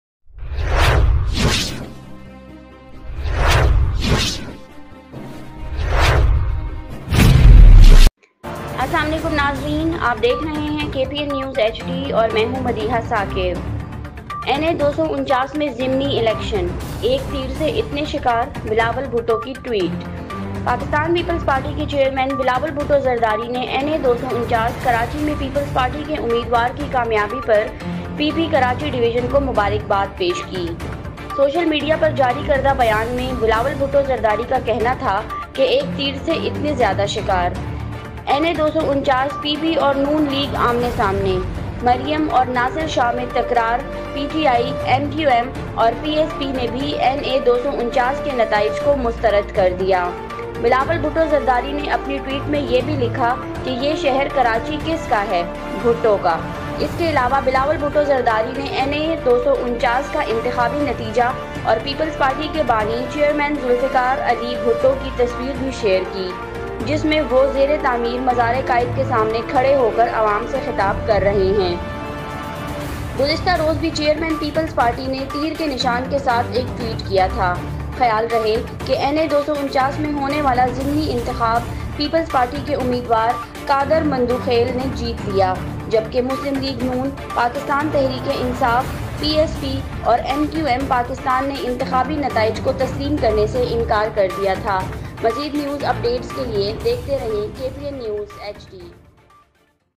NA-249 by-election# so many victims with one# arrow, Bilawal's tweet Pakistan Peoples party.
این اے #249 ضمنی الیکشن: ایک تیر سے اتنے شکار، بلاول کی ٹویٹ#پاکستان پیپلز پارٹی کے چیئرمین بلاول بھٹو زرداری نے این اے #249 کراچی میں پیپلز پارٹی کے امیدوار کی کامیابی پر پی پی کراچی ڈویژن کو مبارکباد پیش کی ہے۔
این اے 249: پی پی اور (ن) لیگ آمنے سامنے، مریم اور ناصر شاہ میں تکرارپی ٹی آئی، ایم کیو ایم اور پی ایس پی نے بھی این اے 249 کے نتائج مسترد کردیے#بلاول بھٹو زرداری نے اپنی ٹوئٹ میں یہ بھی لکھا کہ یہ شہر کراچی کس کا؟ بھٹو کا۔اس کے علاوہ بلاول بھٹو زرداری نے این اے 249 کا انتخابی نتیجہ اور پیپلز پارٹی کے بانی چیئرمین ذوالفقار علی بھٹو کی تصویر بھی شیئر کی جس میں وہ زیر تعمیر مزار قائد کے سامنے کھڑے ہو کر عوام سے خطاب کر رہے ہیں
گزشتہ روز بھی چیئرمین پیپلز پارٹی نے تیر کے نشان کے ساتھ ایک ٹوئٹ کیا تھ
خیال رہے کہ این اے #249 میں ہونے والا ضمنی انتخاب پیپلز پارٹی کے امیدوار قادر مندوخیل نے جیت لیا ہے جبکہ مسلم لیگ ن، پاکستان تحریک انصاف، پی ایس پی اور ایم کیو ایم پاکستان نے انتخابی نتائج کو تسلیم کرنے سے انکار کر دیا ہے۔